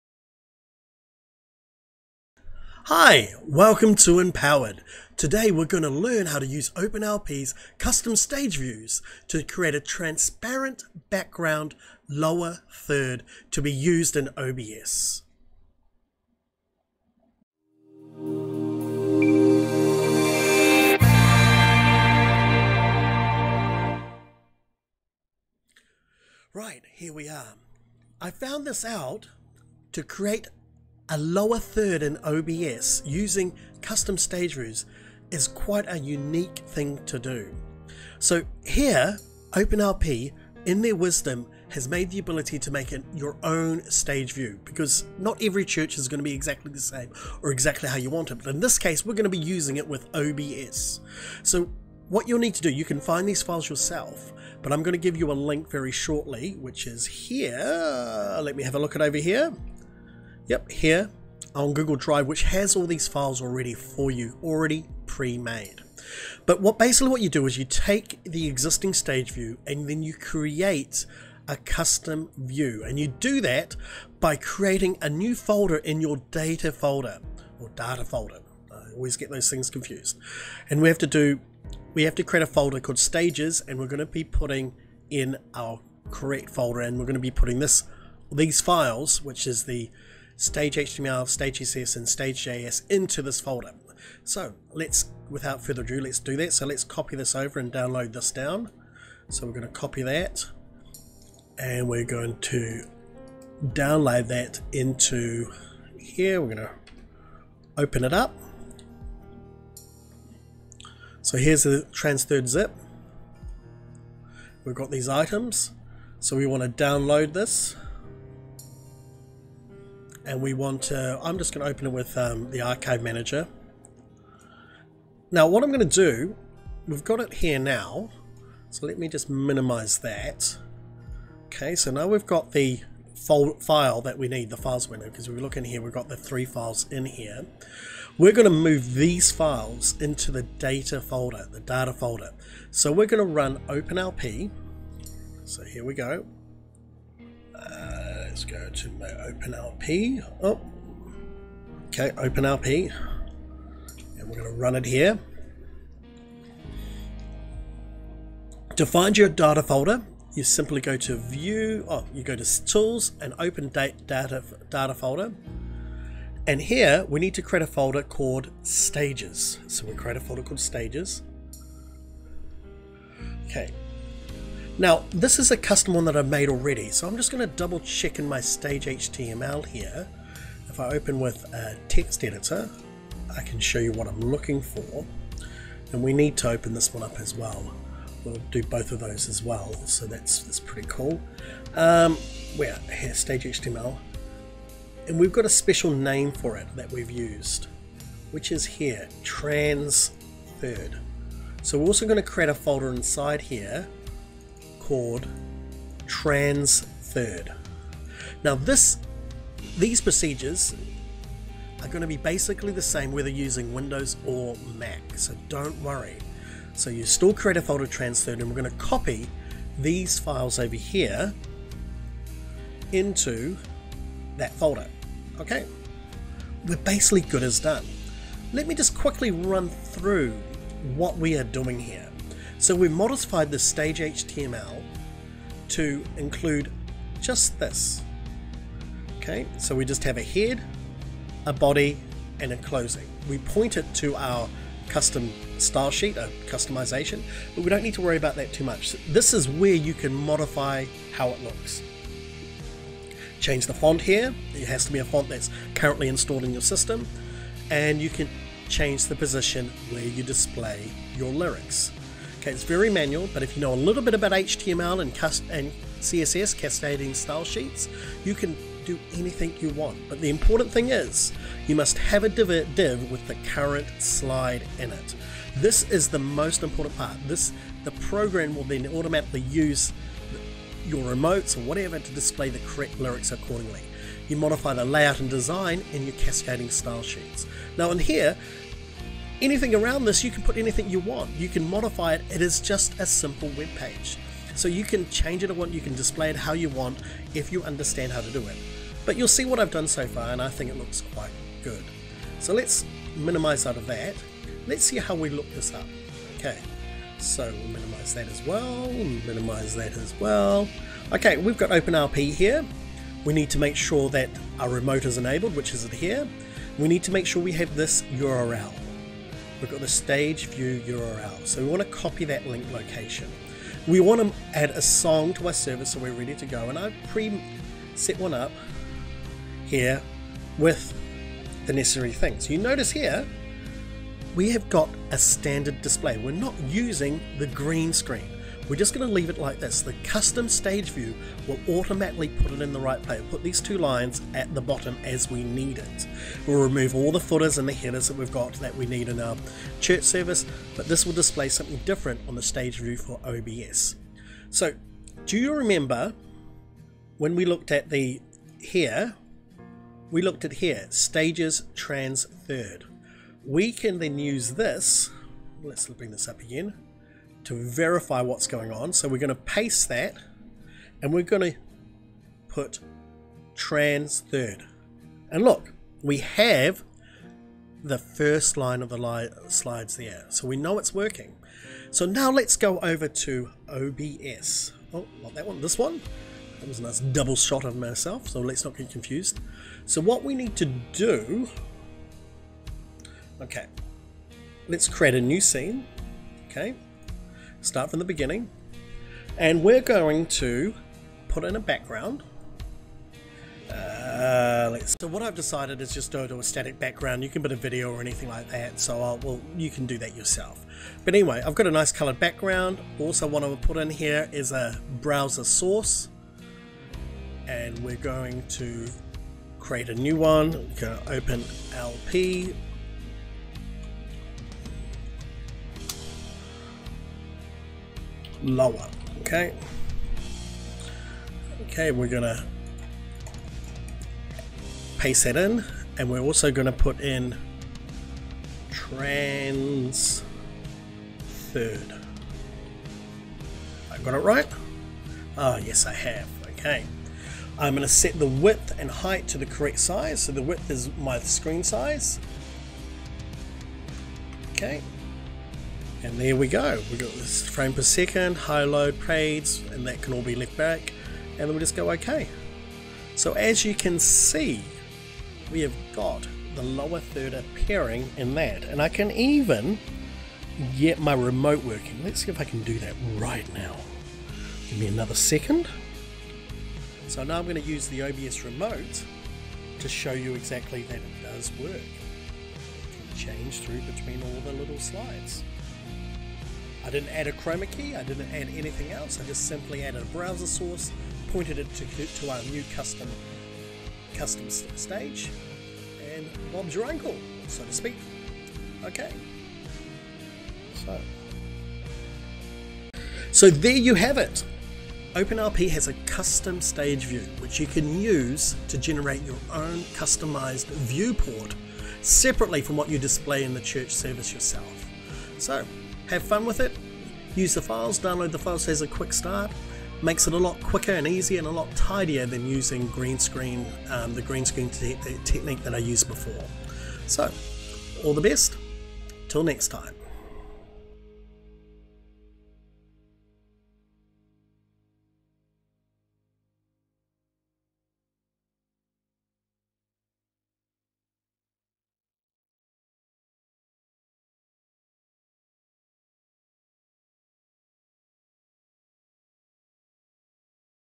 OpenLP Custom Stages with OBS
Today we use OpenLP's custom stage view to create a transparent lower third we can use in OBS.

OpenLP has a great feature called custom stage views. With this you can generate custom outputs that can be viewed via a web-page. This is quite flexable and only limited to your imagination.

OpenLP
My custom stage view which you can find here

updated

You take this and put the contents in the 'stages' folder that you create in the 'data' directory of your OpenLP installation.

In OBS use a 'Browser Source' and point to the stage url appending the custom stage view folder.

Check the video for a detailed explanation.